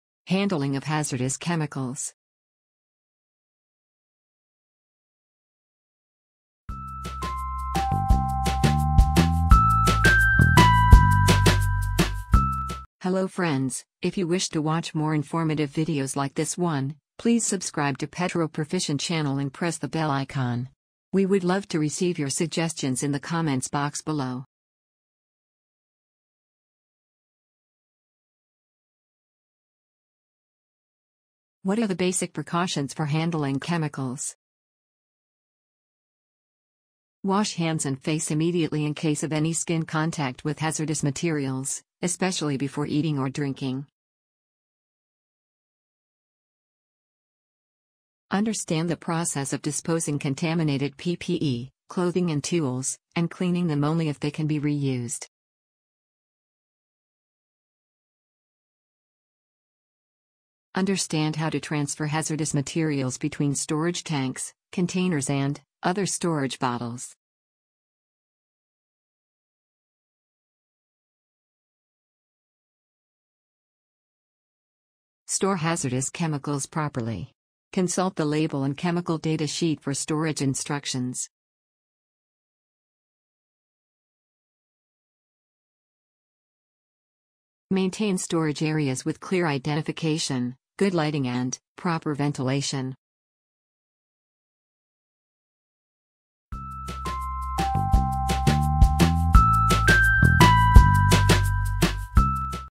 5 Chemical Hazards | Handling of Hazardous Chemicals
What are the basic precautions for handling chemicals ? This video explains the same.